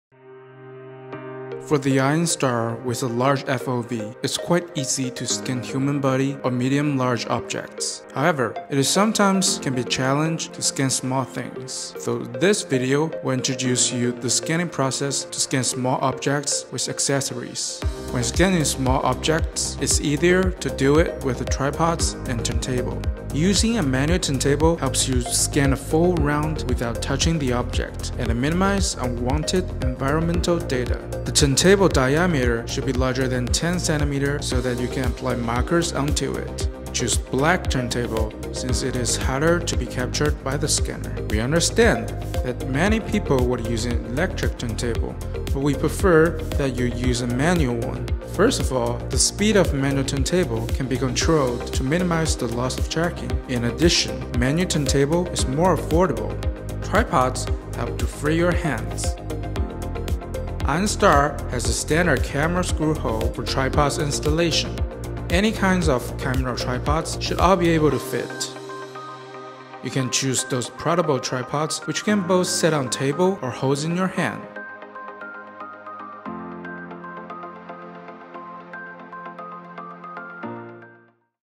Einstar scanning tips: Turntable&Tripod
This video we introduce you the scanning process to scan small objects with accessories turnble and tripod.